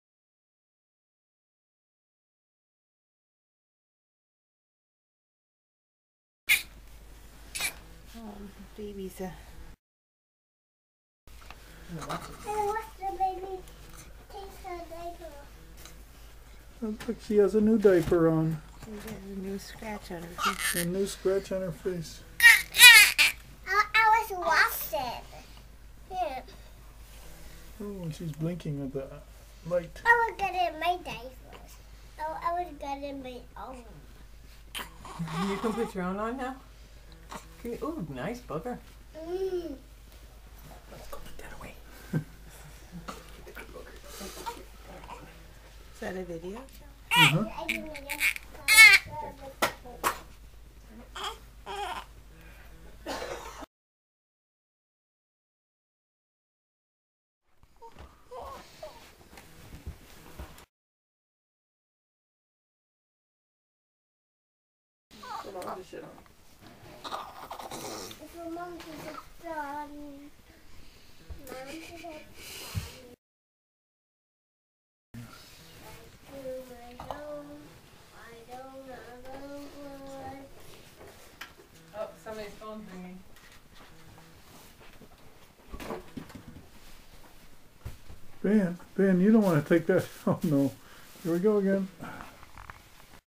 Eleanor Pilling Chappelear day 2
The first day home of Eleanor's life, March 1 2008